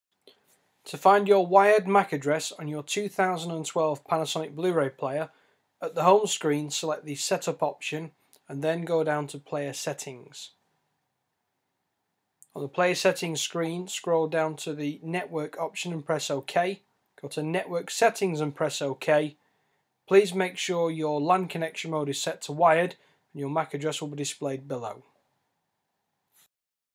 How to Find Your Wired MAC Address 2012 Panasonic Blu-ray Player
This Video will show you how to find your Wired MAC Address on a 2012 Panasonic Blu-ray Player.